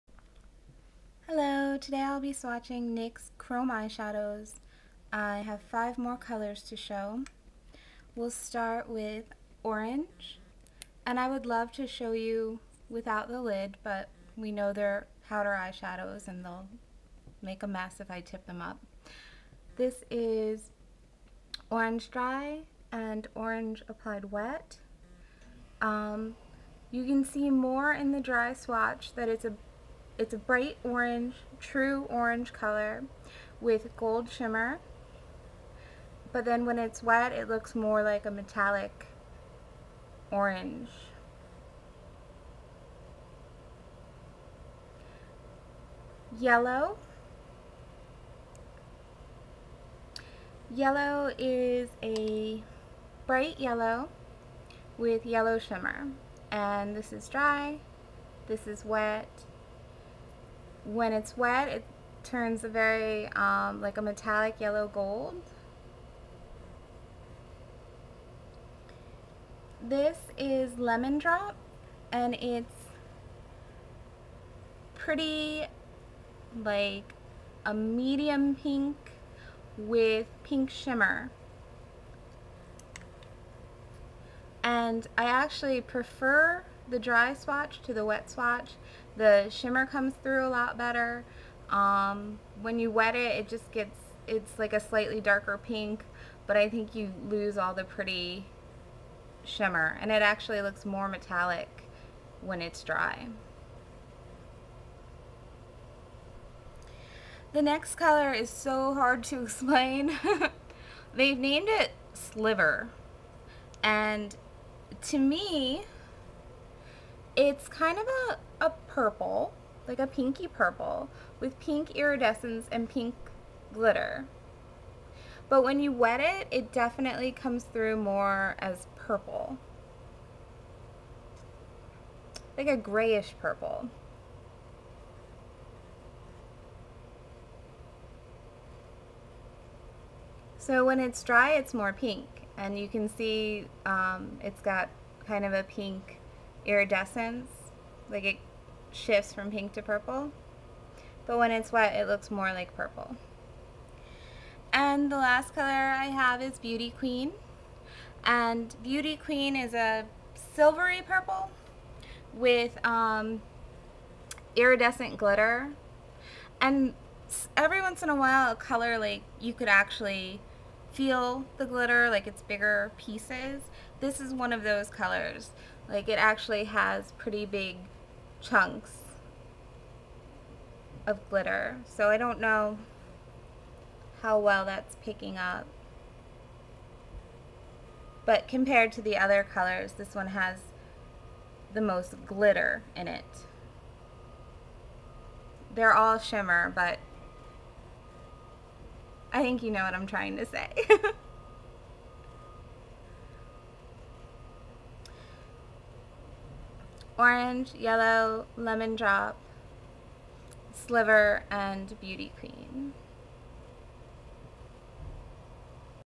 NYX Chrome Shadows II
Orange, Yellow, Lemon Drop, Sliver, Beauty Queen swatched wet and dry, Enjoy! I love em!